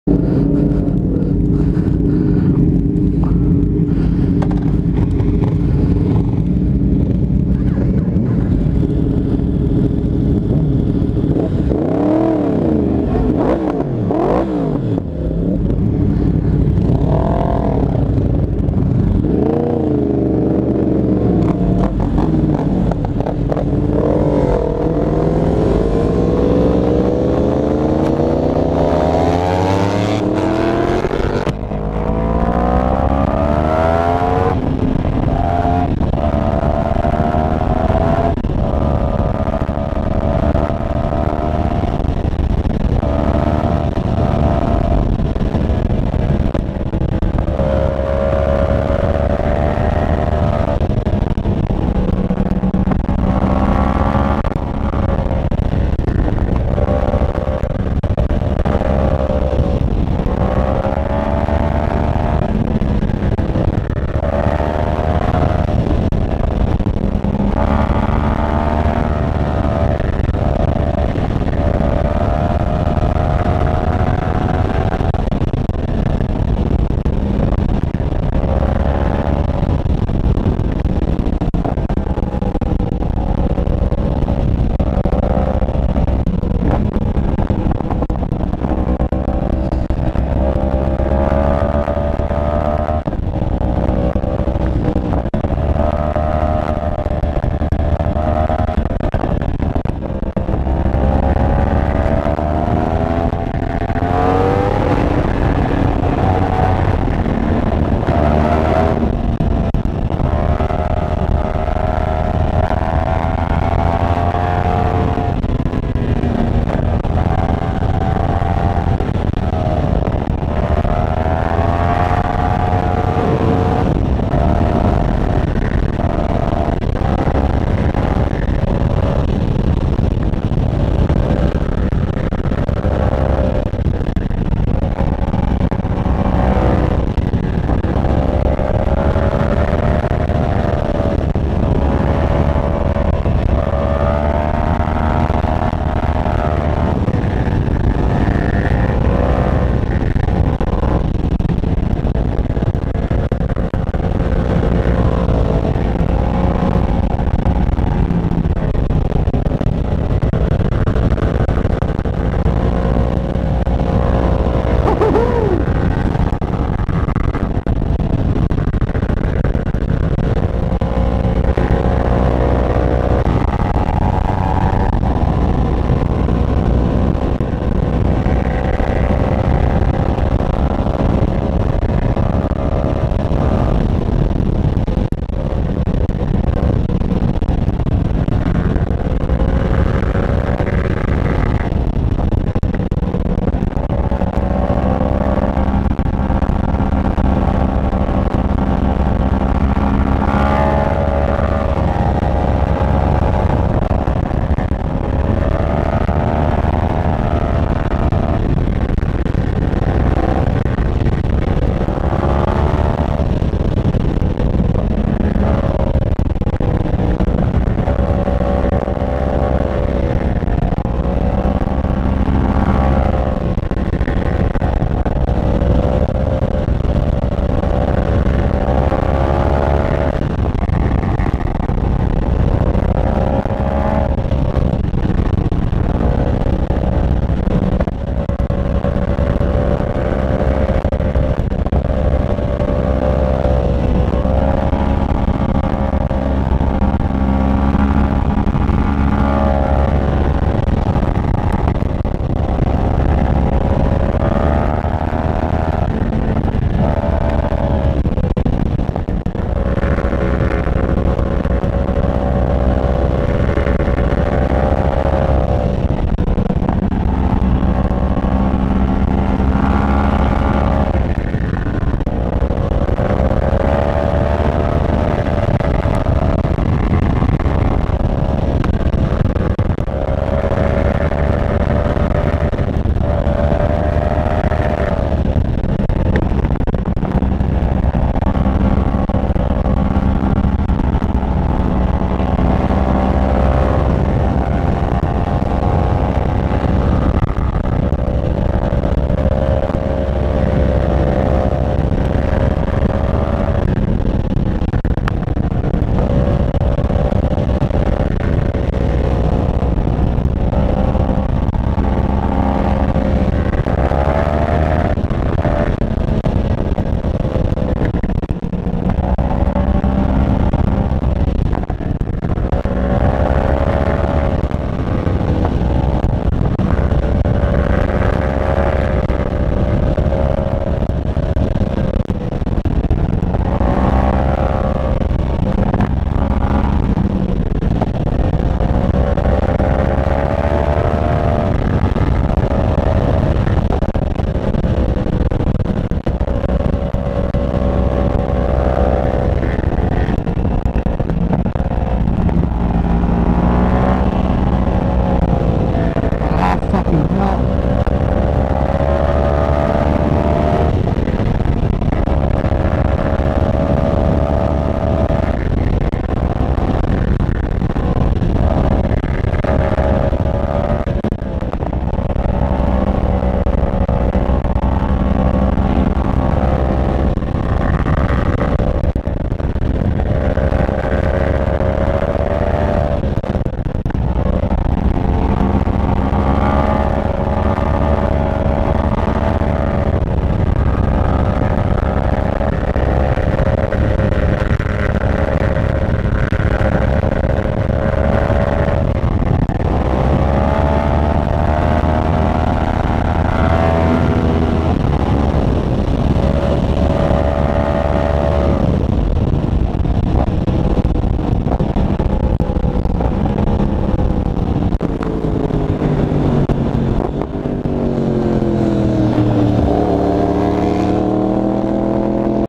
Yamaha R1 chasing “STREET ROSSI” on his Yamaha R3 Angeles Forest Highway !! Pure ADRENALiNE!
Street Rossi shows this R1 that size doesn’t matter

Gopro onboard the Yamaha R1

Location is Angeles National Forest 🌳